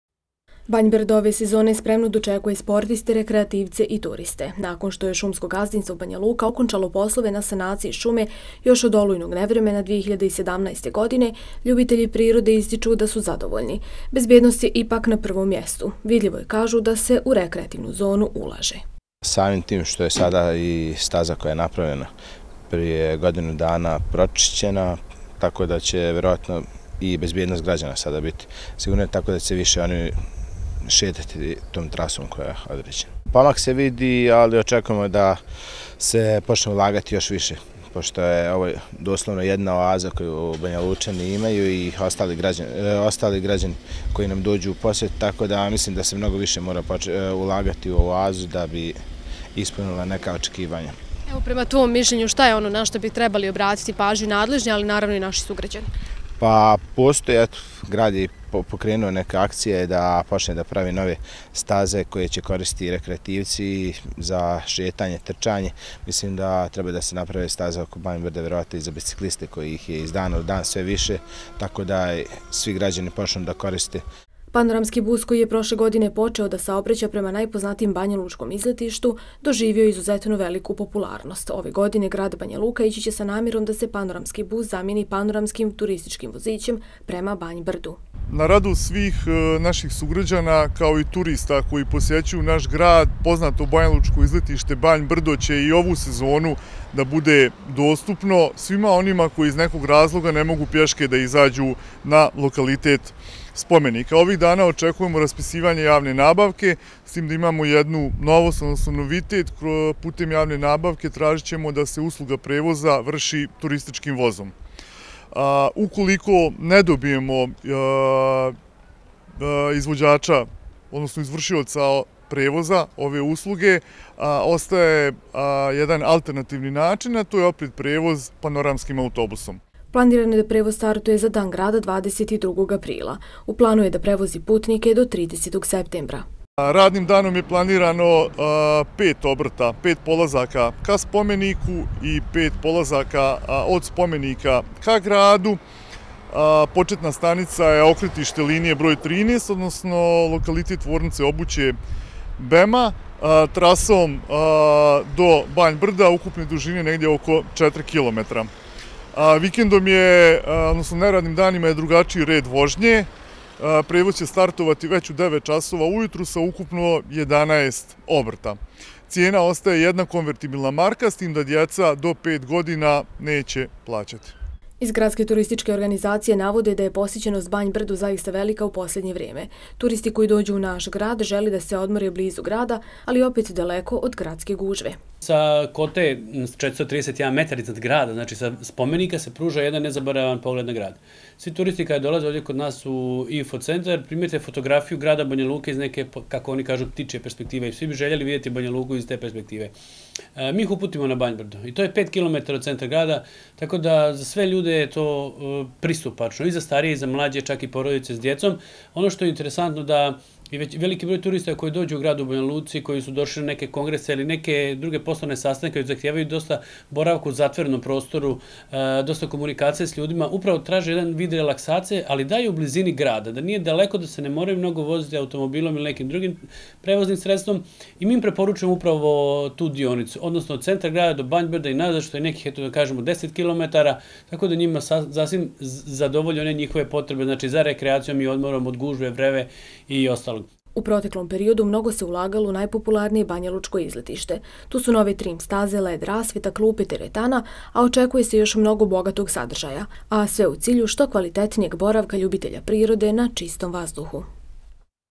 BL HRONIKA 19 03 19 - BANJ BRDO
Najpopularnije i najpoznatije banjalučko izletište spremno će dočekati sve zaljubljenike u prirodu, brojne turiste ali i sve rekreativce željne odmora van grada. Ulagalo se u prethodnom periodu, pa su tako svi sportisti i rekreativci dobili bolje uslove za boravak na otvorenom. I ove godine, saobraćaće mini bus ili panoramski vozić.